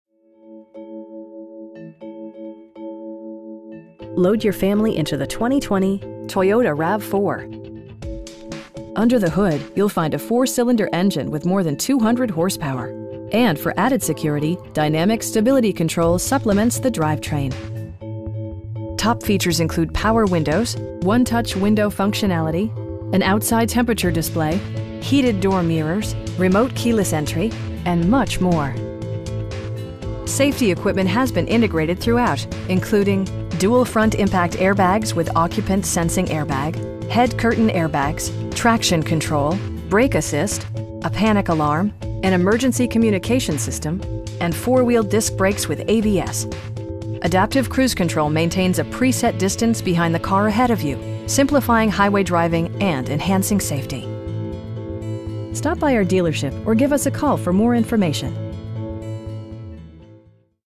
2020 Toyota RAV4 XLE in Arlington, VA 22207
Koons Arlington Toyota
4045 Lee Hwy

Load your family into the 2020 Toyota RAV4. Under the hood you'll find a 4 cylinder engine with more than 200 horsepower, and for added security, dynamic Stability Control supplements the drivetrain. Top features include power windows, 1-touch window functionality, an outside temperature display, front bucket seats, tilt and telescoping steering wheel, heated door mirrors, remote keyless entry, and much more. Safety equipment has been integrated throughout, including: dual front impact airbags with occupant sensing airbag, head curtain airbags, traction control, brake assist, a panic alarm, an emergency communication system, and 4 wheel disc brakes with ABS. Adaptive cruise control maintains a pre set distance behind the car ahead of you, simplifying highway driving and enhancing safety. You will have a pleasant shopping experience that is fun, informative, and never high pressured. Stop by our dealership or give us a call for more information.